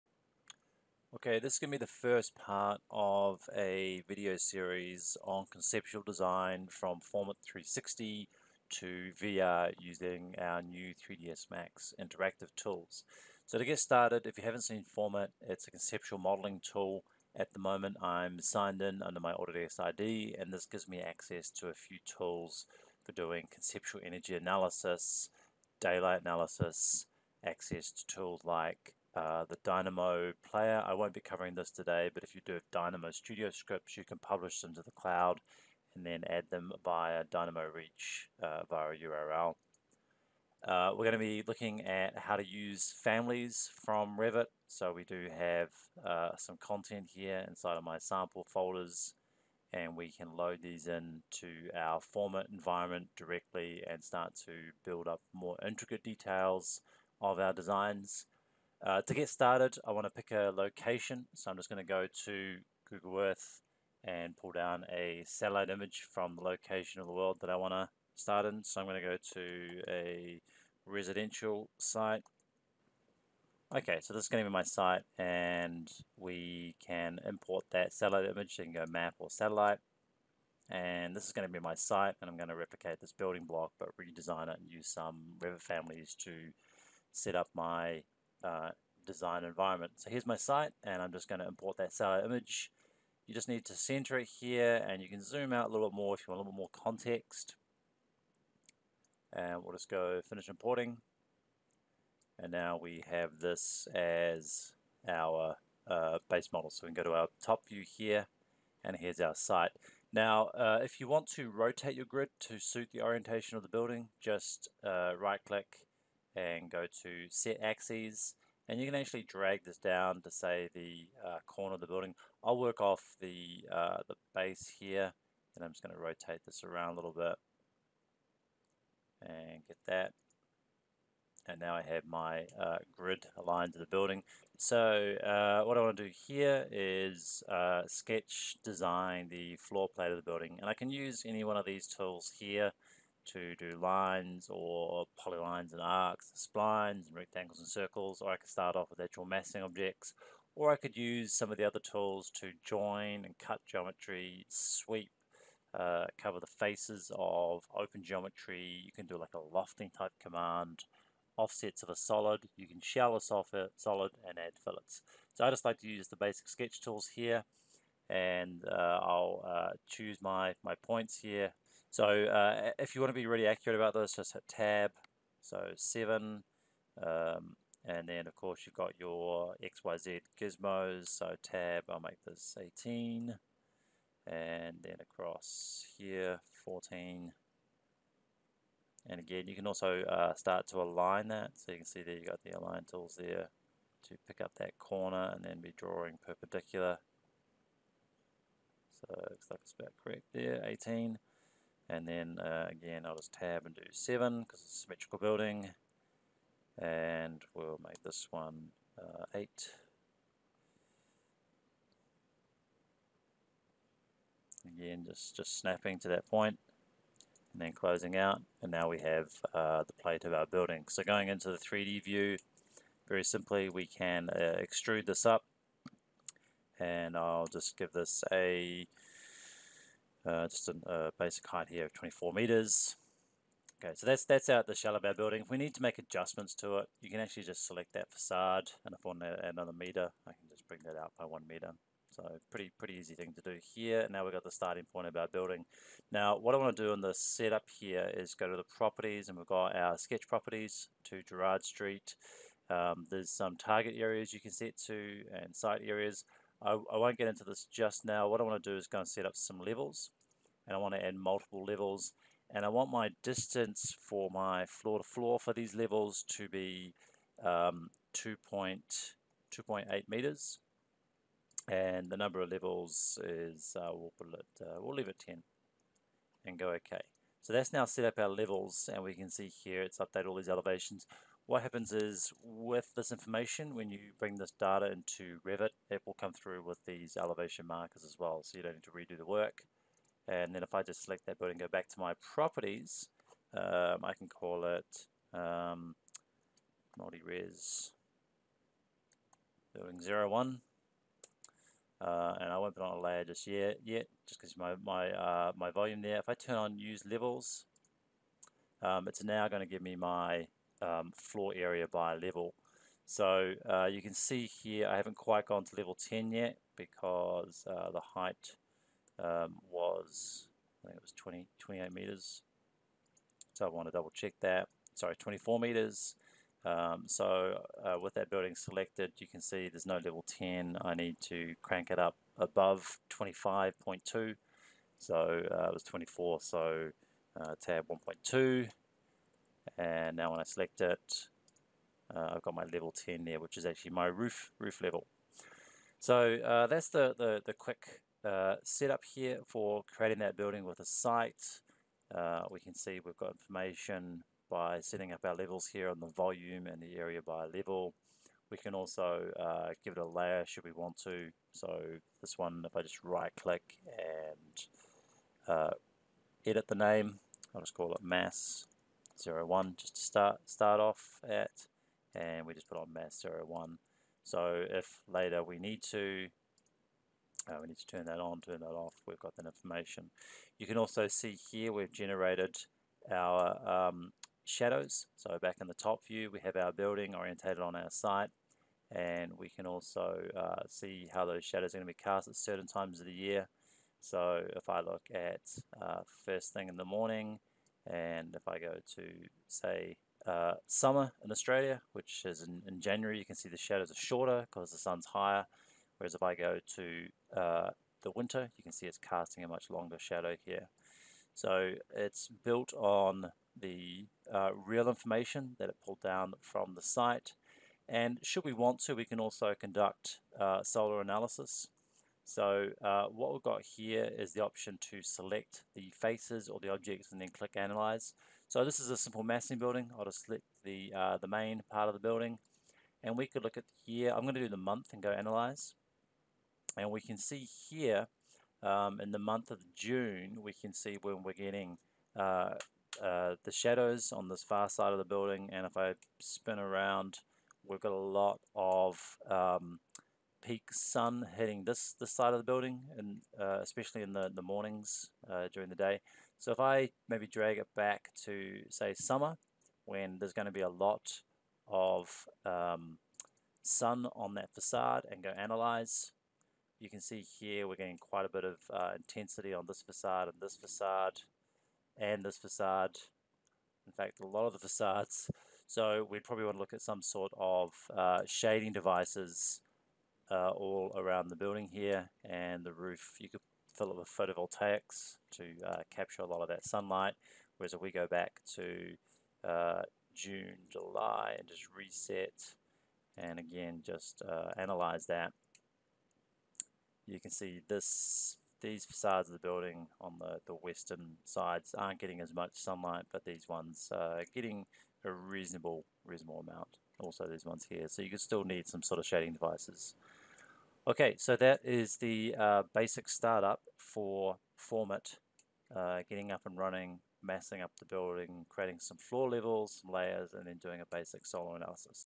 FormIt 360 getting started
In this video you will learn you to set up a basic FormIt massing model and use the daylight analysis tool